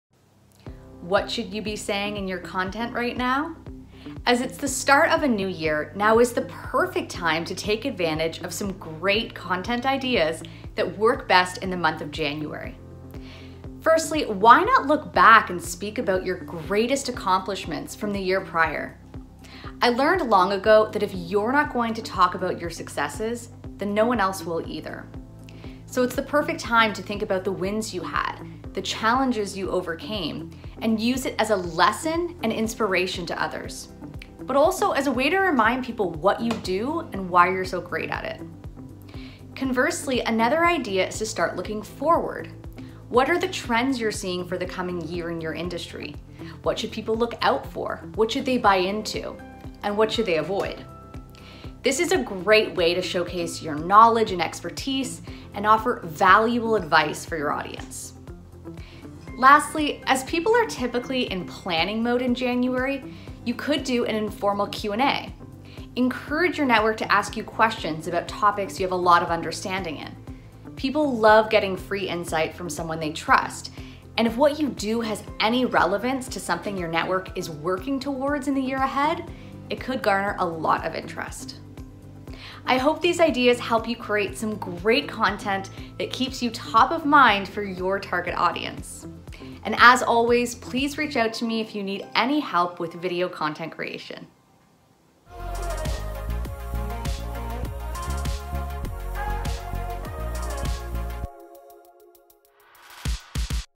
Content Ideas for January
Not sure what to say? You know you should be putting yourself out there on LinkedIn more, but you hesitate in terms of what to talk about.

Trust me I've been there. It's been debilitating. Debating what is "worthy" of posting about.

In pushing myself to post more frequently in the last year I discovered that I have a lot to share and I'm sure you do too.

Here are 3 easy and quick to do ideas that are perfect for this time of year.

***And taking a bit of my own advice... let me know in the comments below if you have any questions when it comes to content marketing, video production, performance techniques - I'd be happy to try and answer them in an upcoming post.